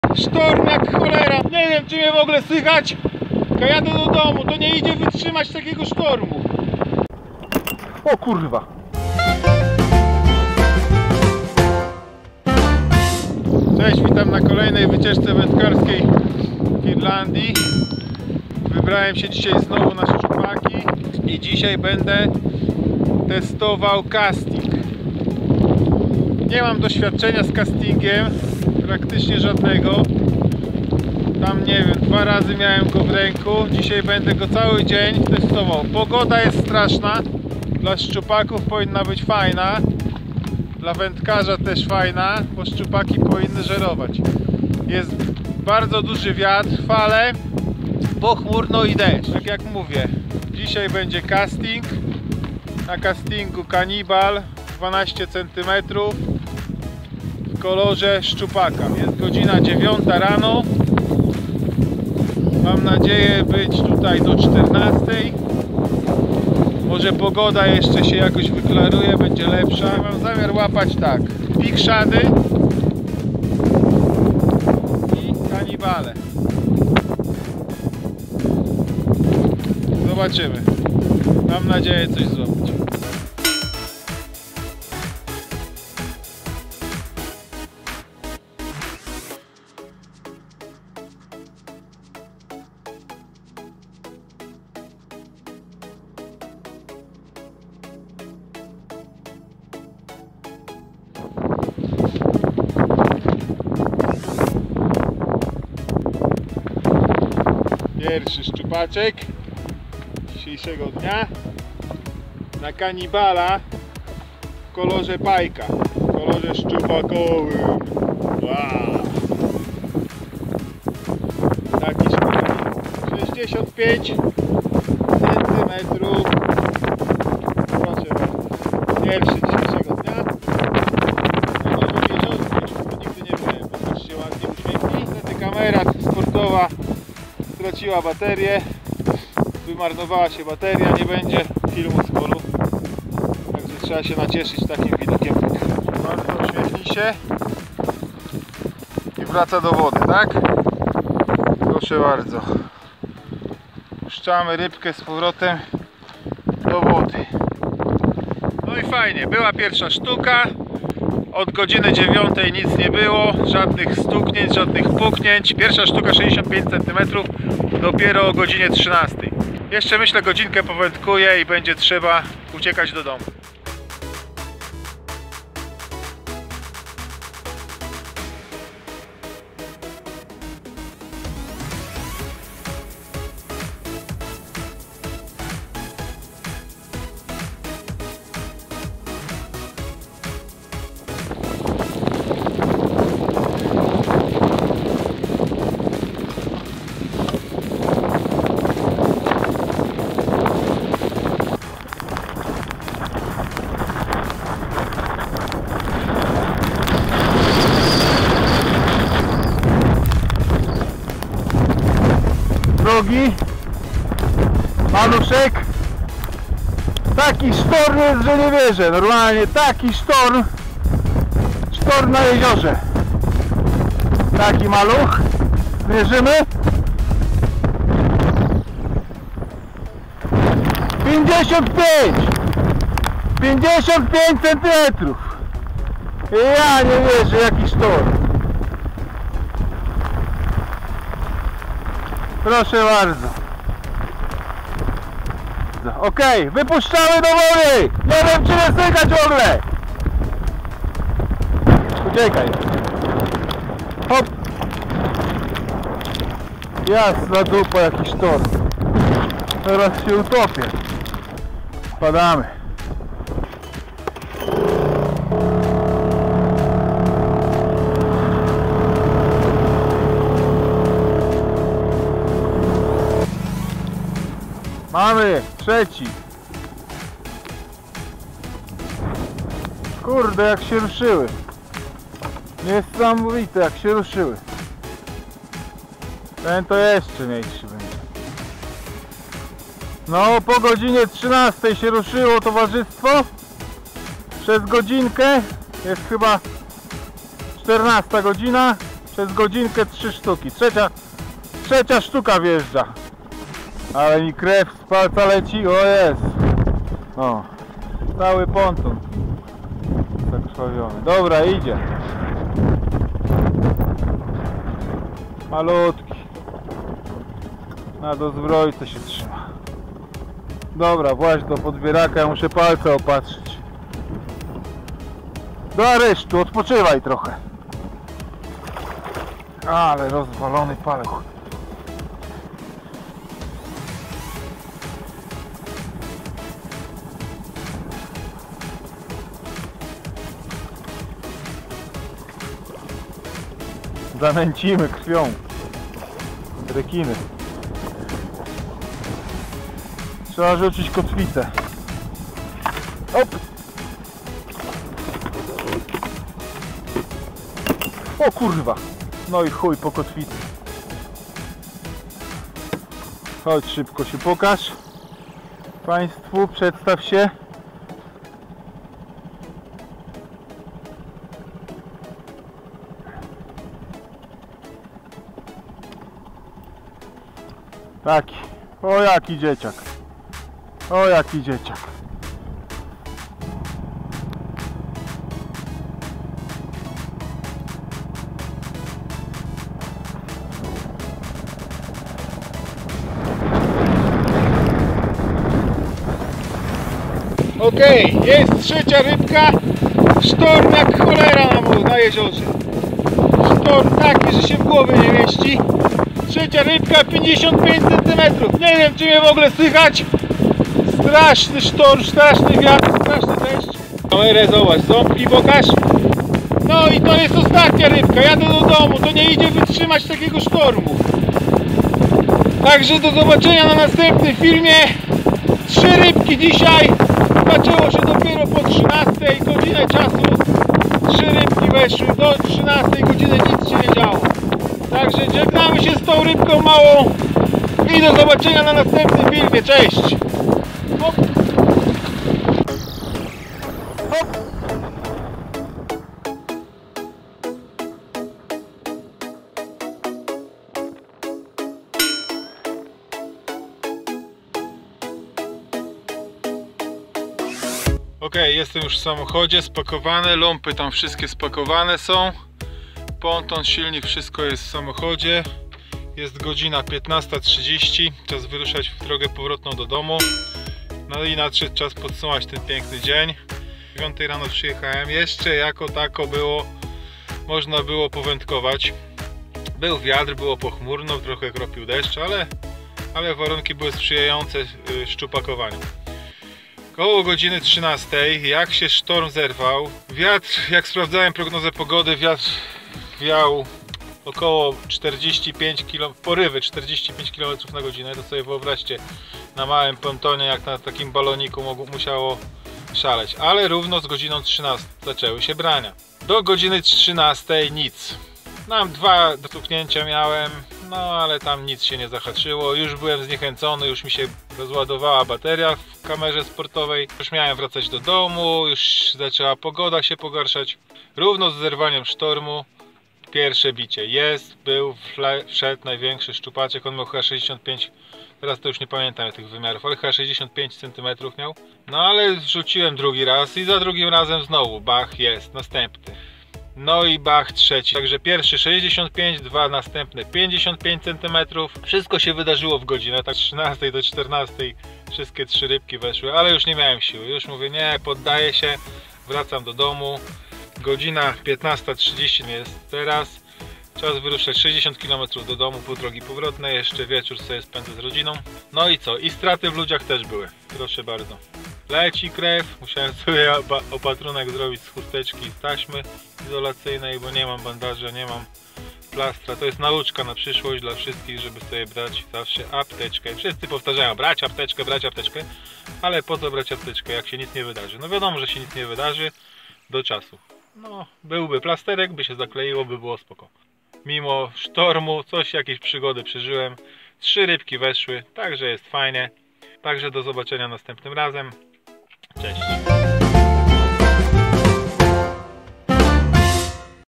#10 - Szczupaki | Sztorm na jeziorze
Wędkowanie z przygodami, nagłe załamanie pogody na jeziorze, silny wiatr, ulewa, pierwszy dzień z castingiem i utopiona "kotwica".
Zapraszam na film ... szczupaki też są :)